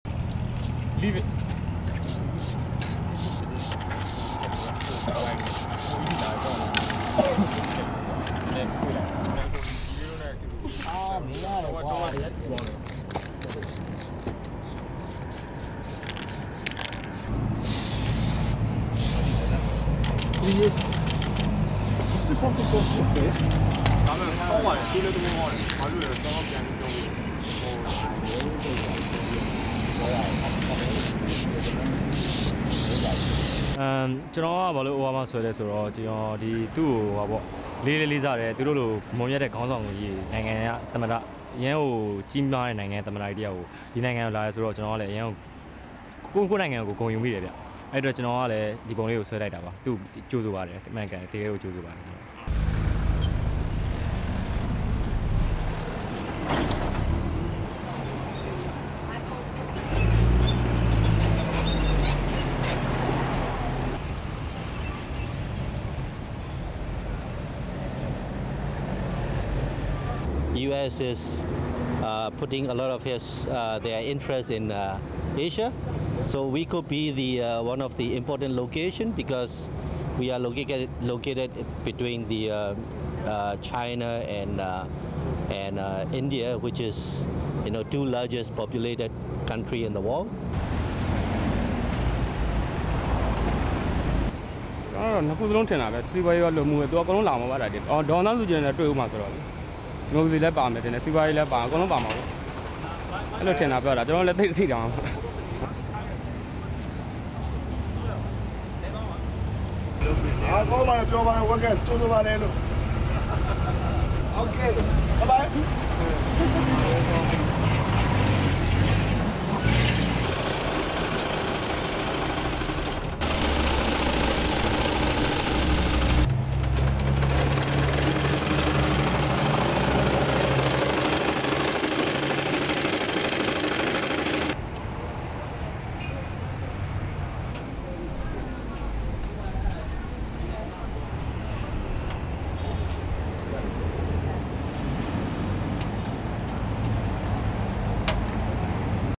Artist decorates the town with graffiti to welcome the US President
++NIGHT SHOTS++
1. Various of graffiti artist Arker Kyaw working on image of US President Barack Obama on wall, reading: (English) "Welcome Obama"
2. SOUNDBITE: (Burmese) Arker Kyaw, graffiti artist:
"I drew Obama's image because he is the president of a great country. I am also proud that he is coming to visit our country. I warmly welcome him."
++DAY SHOTS++
3. Various of completed graffiti, vehicles passing in street
4. Pan from man taking photograph to graffiti on wall
5. SOUNDBITE: (English) Soe Min Aung, Labour union activist:
''The US is putting a lot of its interest in Asia. So we could be one of the important locations, because we are located between China and India, which are the two most largely populated countries in the world."
6. Mid of people looking at Obama welcome graffiti from passing car
7. SOUNDBITE: (Burmese) Moe Thaw, 25, mechanic, Vox pop:
"I think (Obama is coming here) for both reasons, economic and social. He (Obama) will meet Aung San Suu Kyi. And maybe this visit will also be about democracy, apart from the economy. But I really do not know."
8. Tilt up of Obama's image in graffiti design
9. SOUNDBITE: (Burmese) Tauk Tun, bus driver, Vox pop:
"I am just happy and I welcome him!"
10. Wide of bus operator driving vehicle away
11. Various of woman sewing US flag
12. Close-up of vendor
13. Pan across T-shirts on sale featuring images of Myanmar pro-democracy leader Aung San Suu Kyi and Obama
14. Close-up of Obama image on T-shirt
STORYLINE
A Myanmar artist put the final touches to a piece of street graffiti on Saturday, designed to welcome US President Barack Obama to the country.
Arker Kyaw sprayed an image of the leader onto a wall next to a busy road in Yangon, along with the words "Welcome Obama."
Obama is due to arrive in Myanmar on Monday.
It will be the first time a US president has visited the former pariah state.
"I drew Obama's image because he is the president of a great country. I am also proud that he is coming to visit our country. I warmly welcome him," said Kyaw.
The street artist had to create his graffiti design twice, however, after his original creation was scribbled over with a tin of black paint by a suspected rival artist.
Media coverage of Kyaw's work had led to him being invited to a speech by Obama at the University of Yangon, which has been a seat of opposition since colonial times.
Obama is scheduled to meet opposition leader Aung San Suu Kyi as well as President Thein Sein, who is widely credited with driving the country's recent political and economic reforms.
''The US is putting a lot of its interest in Asia. So we could be one of the important locations, because we are located between China and India, which are the two most largely populated countries in the world," said Soe Min Aung, a labour union activist.
Moe Thaw, a 25-year-old mechanic, added: "I think (Obama is coming here) for both reasons, economic and social. He (Obama) will meet Aung San Suu Kyi. And maybe this visit will also be about democracy, apart from the economy."